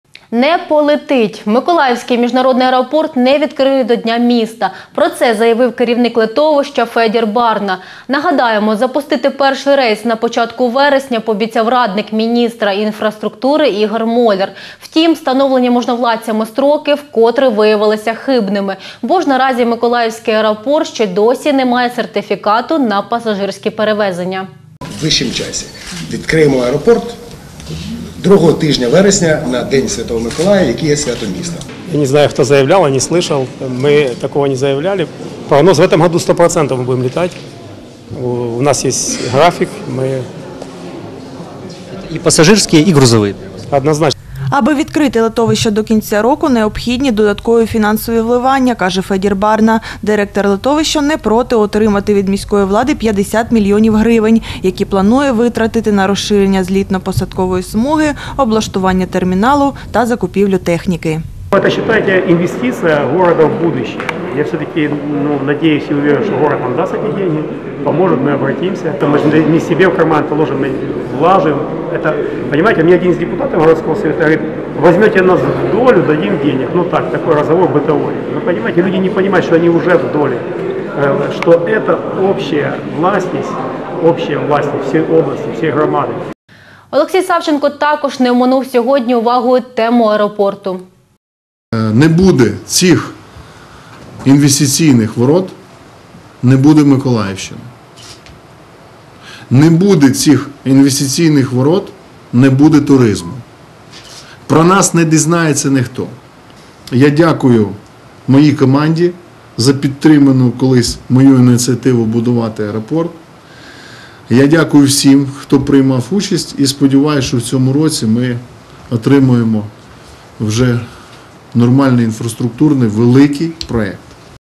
Объектив 31 08 18 Николаевский аэропорт не запустят ко Дню Города
Автор: Лариса Евсеева